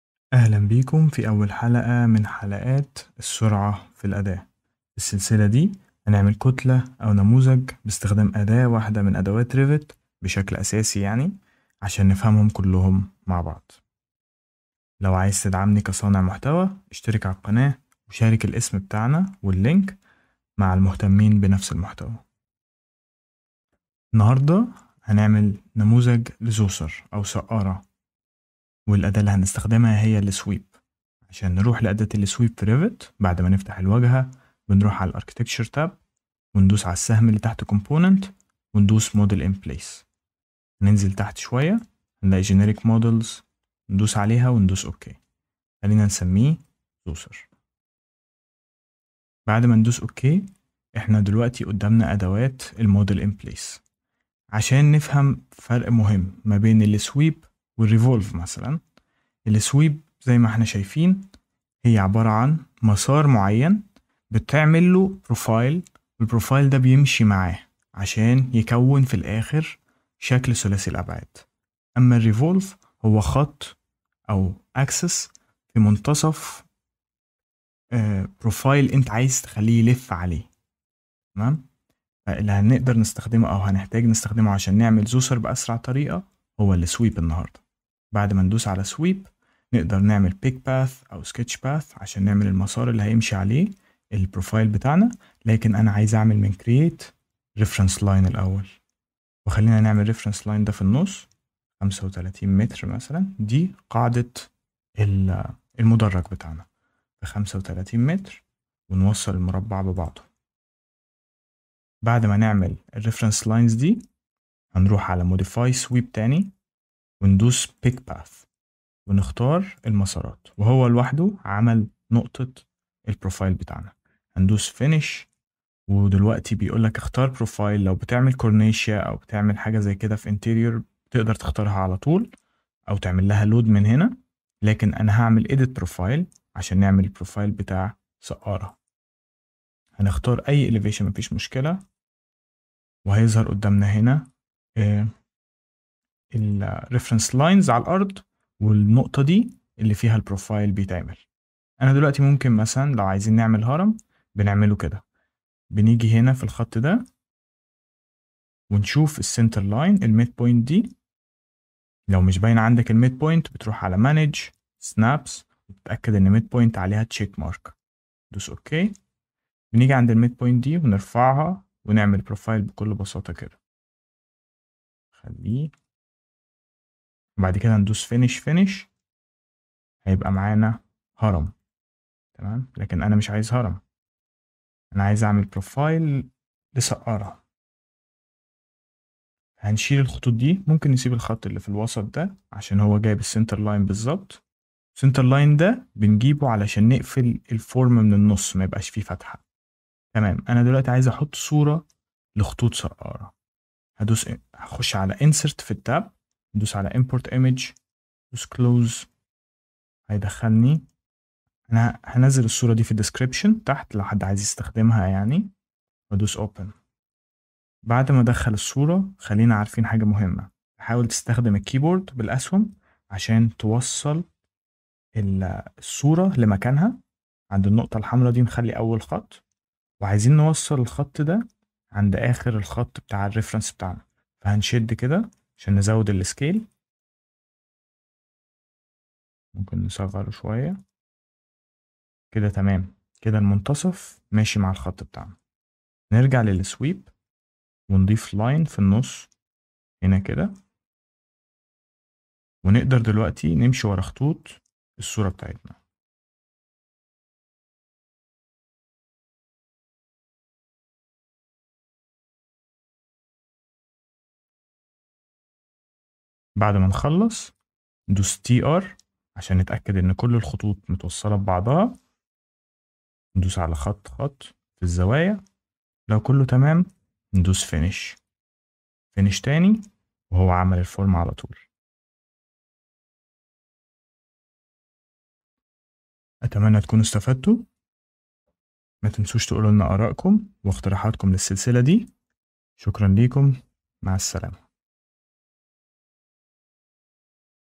هرم سقارة عبر أداة سويب على ريفيت | Sweep in Revit
Project Files ملفات المشروع

Modeling Djoser pyramid to master Sweep tool in Revit